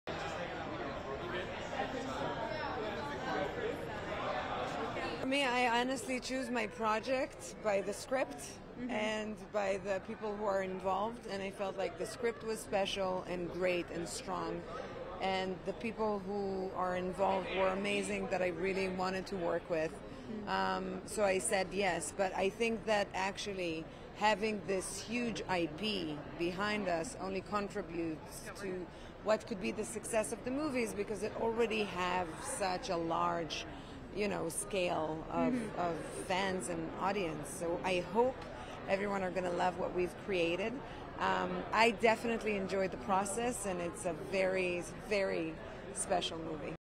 At D23 expo, Gal Gadot discusses live action version of 'Snow White'
ASSOCIATED PRESS
Anaheim, California, 9 September 2022
1. Actors Rachel Zegler and Gal Gadot speak to reporter
2. SOUNDBITE (English) Gal Gadot, actor, "Snow White"
Gadot: "For me, I honestly choose my projects by the script and by the people who are involved, and I felt like the script was special and great and strong and the people who are involved were amazing that I really wanted to work with. So, I said, 'Yes.' But I think that actually having this huge IP behind us only contributes to what could be the success of the movie, because it already has such a large, you know, scale of fans and audience. So, I hope everyone are going to love what we've created. I definitely enjoyed the process and it's a very, very special movie."
AP VIDEO SHOT BY Rick Taber
PRODUCTION: Krysta Fauria and Mike Cidoni Lennox